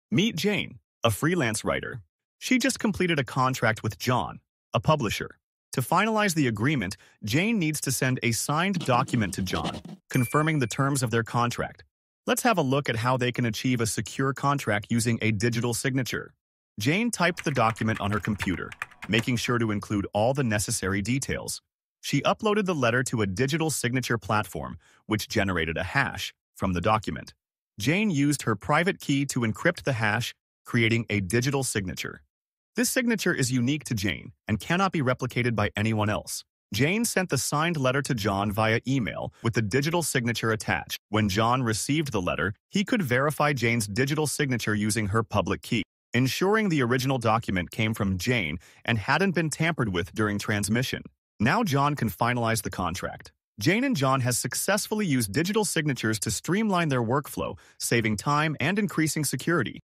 How Digital signatures work
This video explains digital signatures in simple terms, covering:
How they're created
Verification process
Real-world applications
A concise introduction to digital signatures.